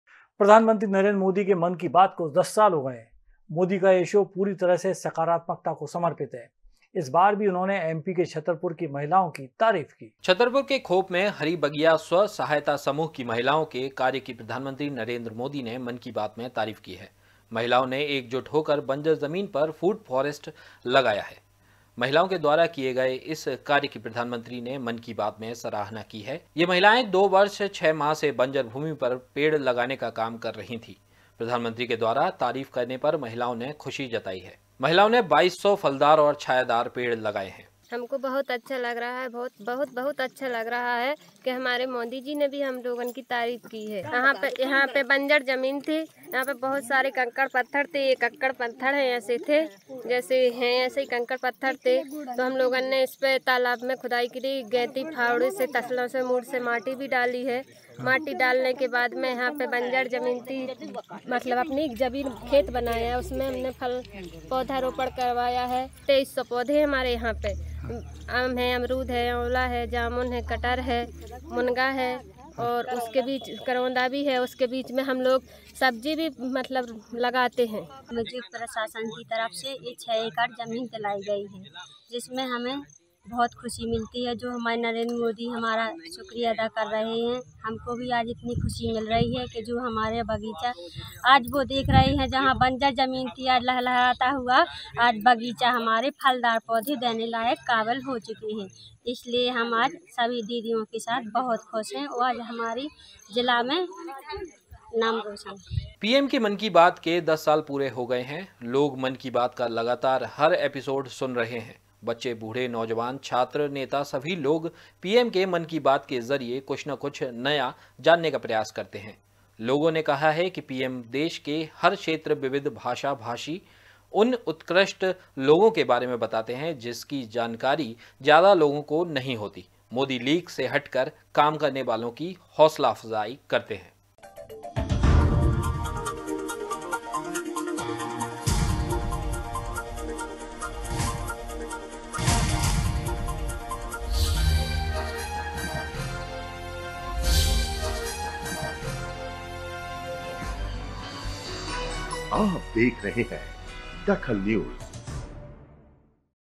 DAKHAL NEWS मोदी ने की छतरपुर की महिलाओं की तारीफ़
मोदी ने की छतरपुर की महिलाओं की तारीफ़
दस सालों से पीएम मोदी कर रहे हैं पॉजिटिव बात